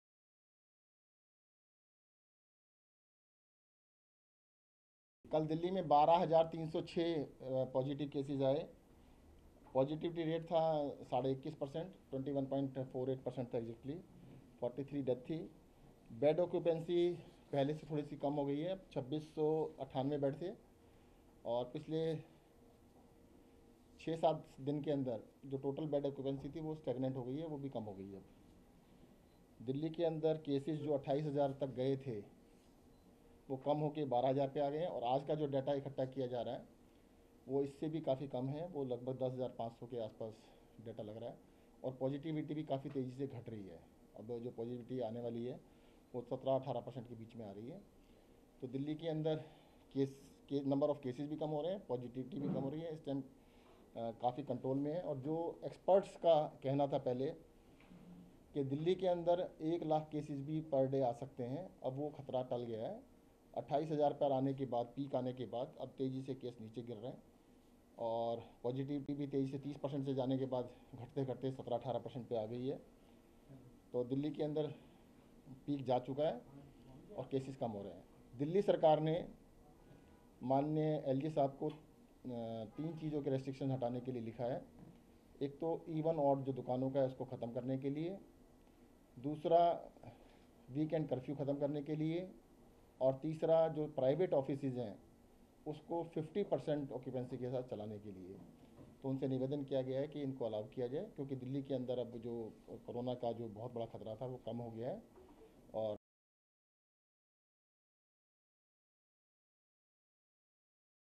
COVID-19: Peak of third wave passed in Delhi, says Satyendra Jain
New Delhi, Jan 21 (ANI): Briefing on the present COVID situation in the national capital, Delhi Health Minister Satyendra Jain on January 21 informed that the cases are decreasing and today the cases are expected to be around 10,500 and the peak of the COVID third wave has passed in the city. “Yesterday there were 12,306 positive cases of COVID in Delhi. The positivity rate was 21.48 per cent and 43 people had died. Cases are decreasing and positivity is also decreasing. It is expected that today the COVID cases will be around 10,500. The peak of COVID has passed in Delhi,” he added. (JA)